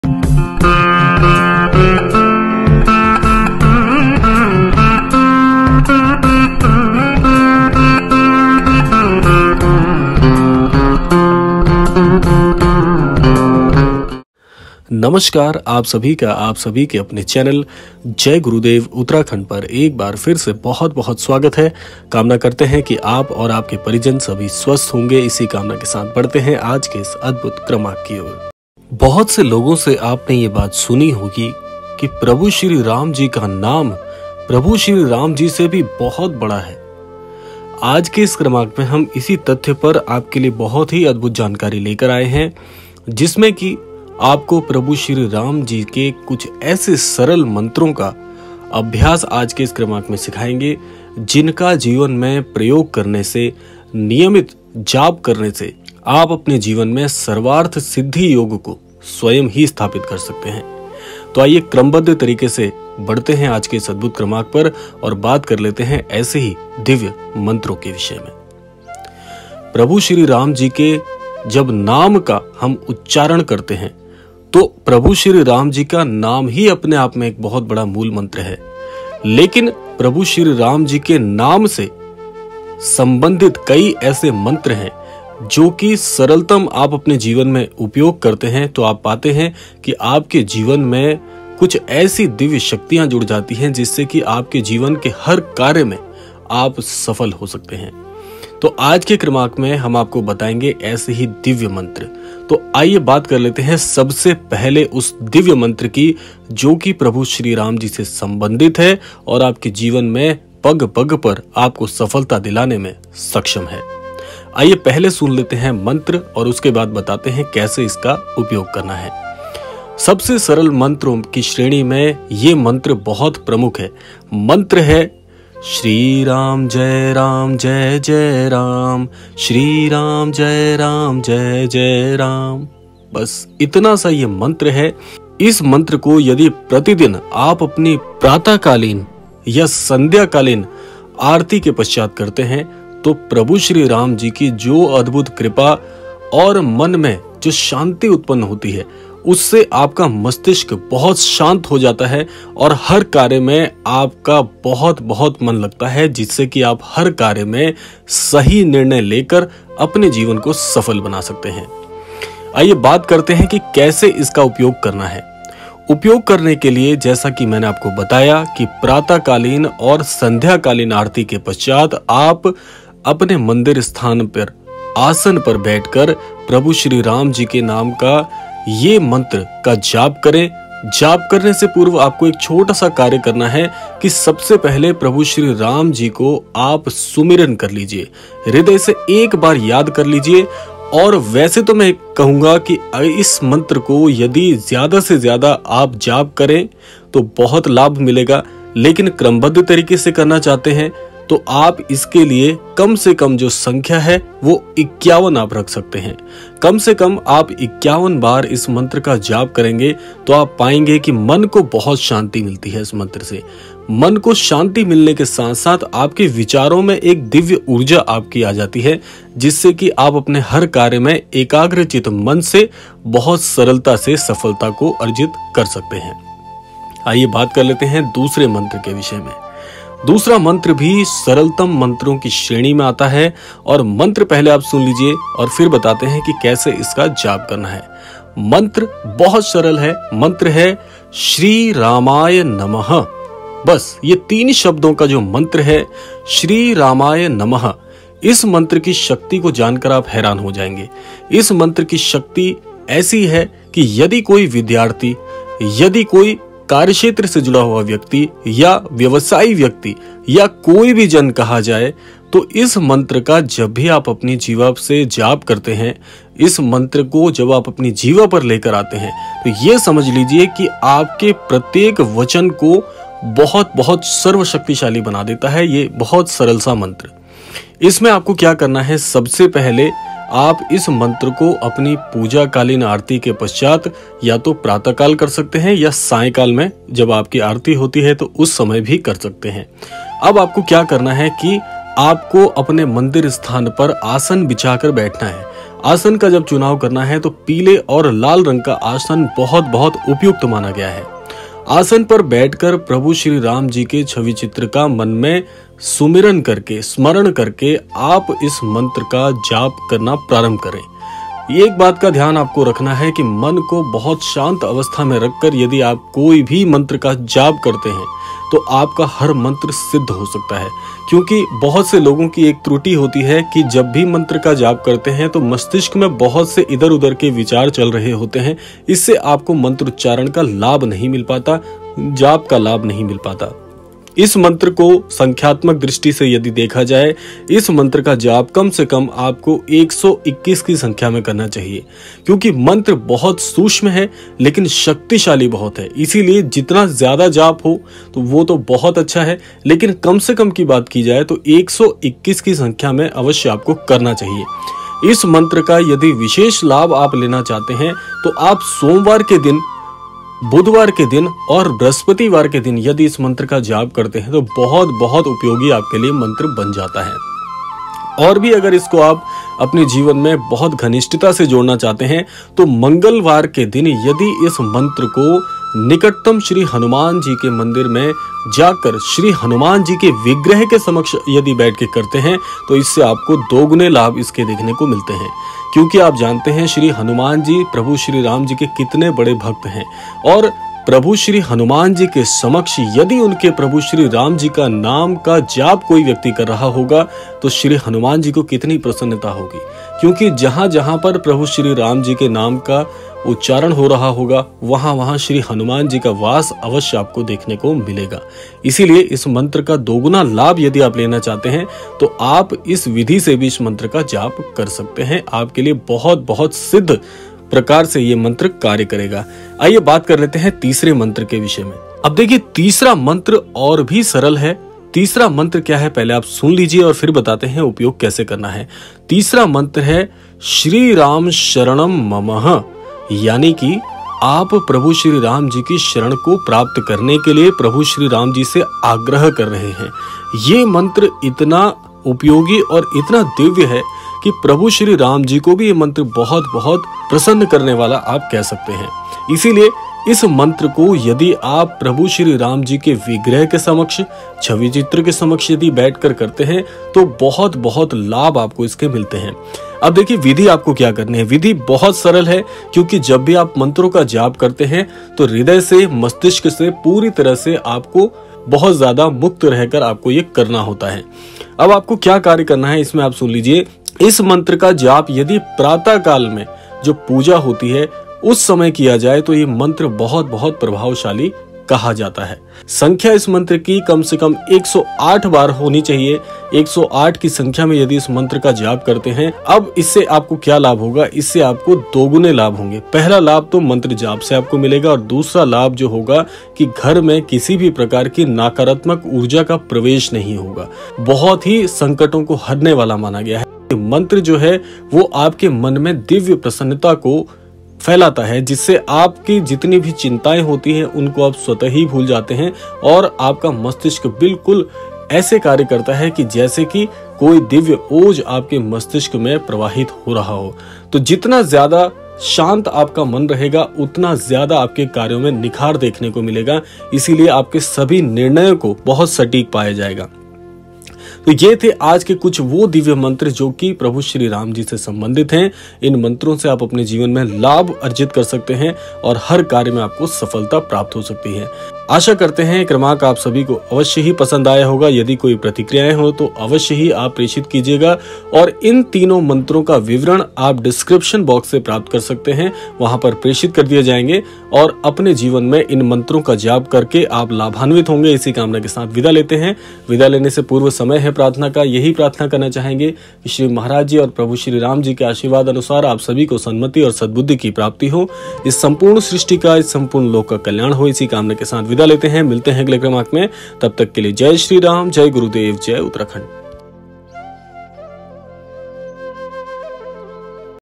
भगवान श्री राम के ये मंत्र रातों रात बना देंगे आपको भाग्यशाली। | Neem Karoli Baba | Miracles Of Love
जयगुरुदेव 🙏
आज के विशेषांक मे जानिए भगवान श्री राम के ये मंत्र रातों रात बना देंगे आपको भाग्यशाली। | पूरी जानकारी के लिए क्रम को पूरा अवश्य देखिएगा।

Regards
Jaigurudev Uttarakhand